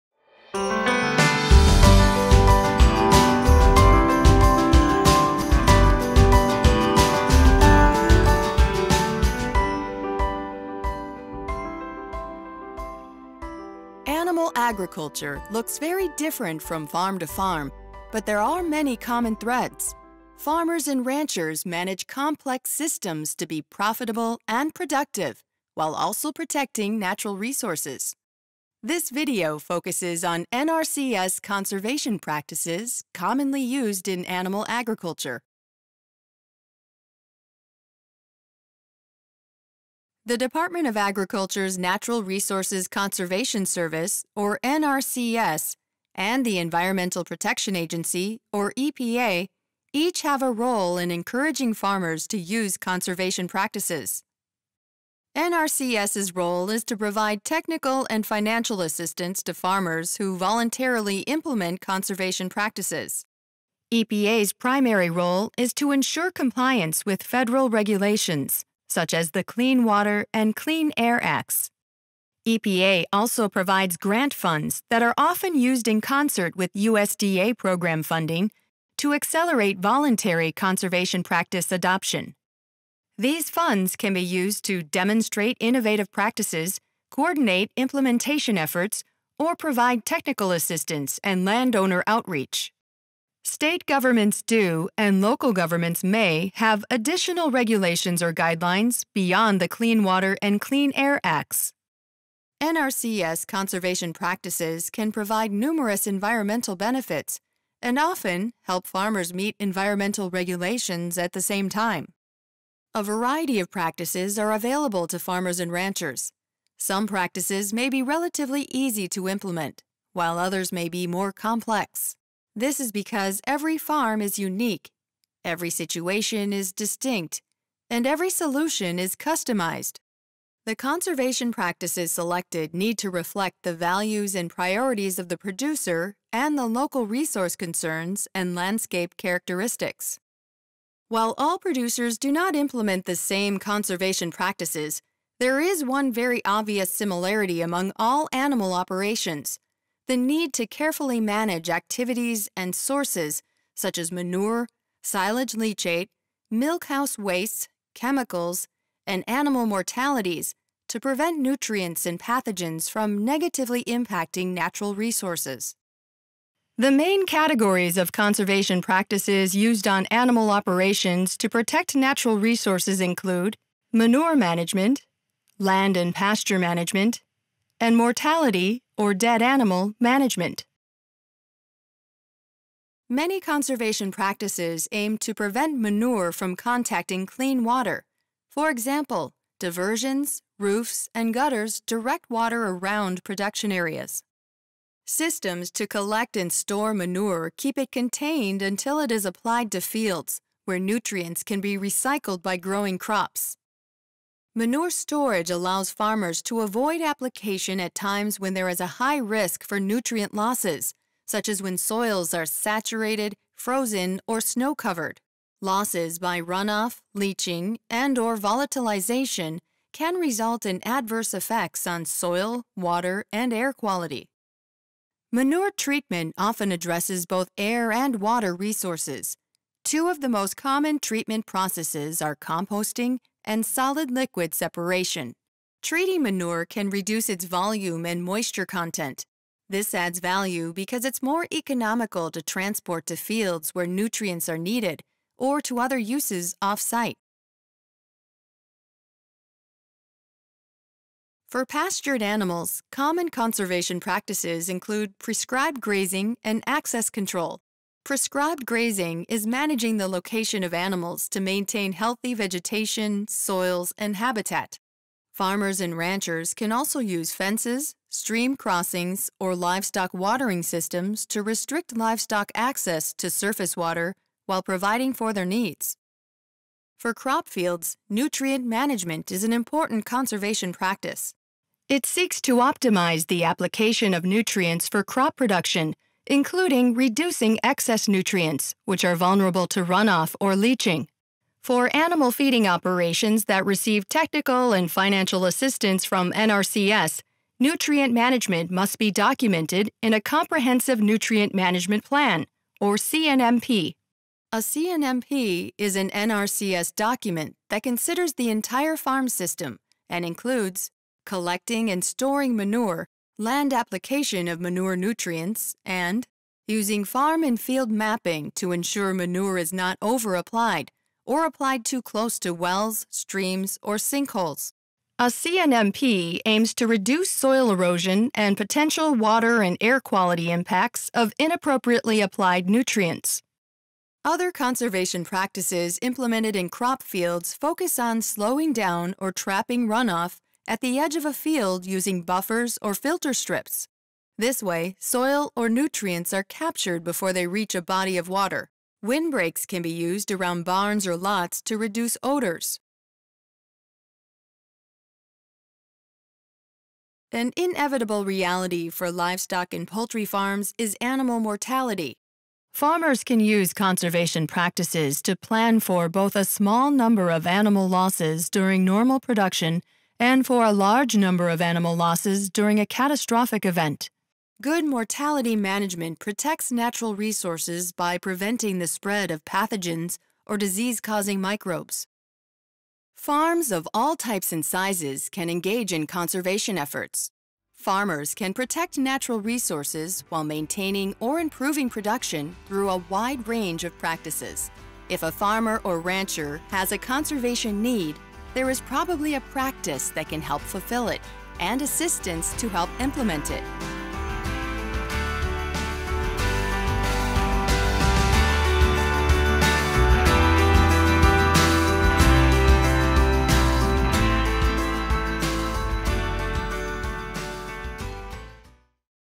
Conservation Practices Commonly Used in Animal Agriculture
This video describes several conservation practices that are used on animal agriculture operations. This includes manure management, mortality management, and land/pasture management.